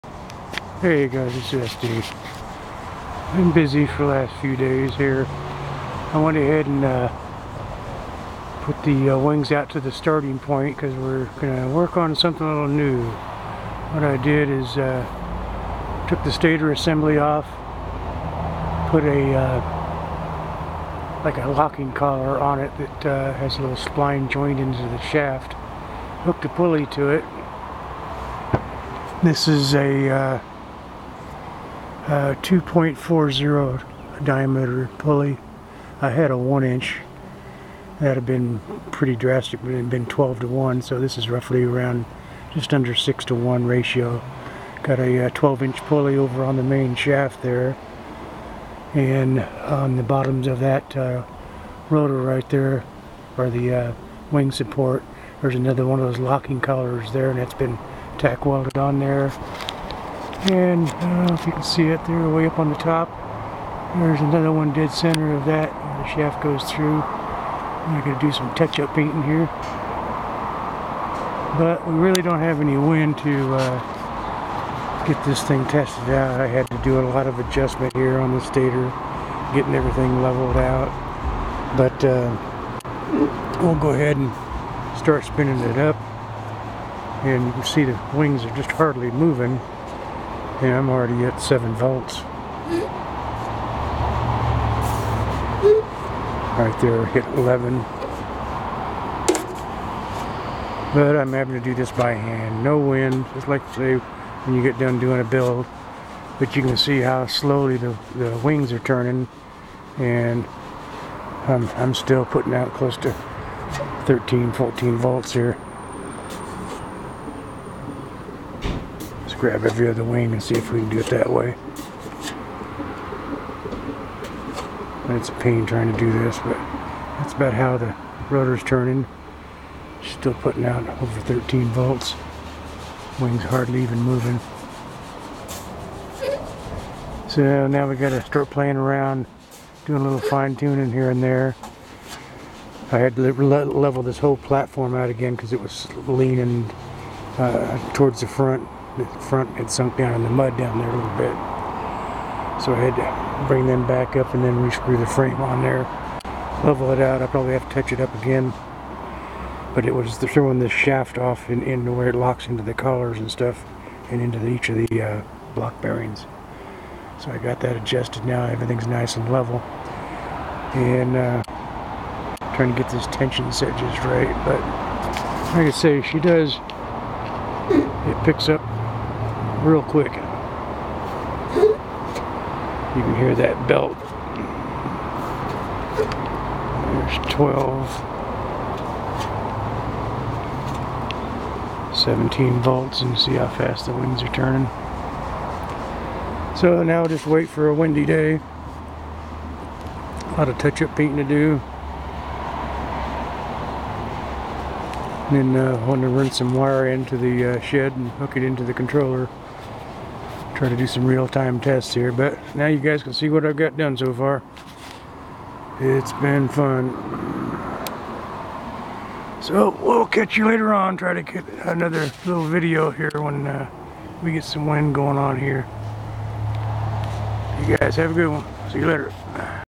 VAWT belt drive set up
I finally got the modification to the vawt drive system done. now just have to wait for some decent wind for further testing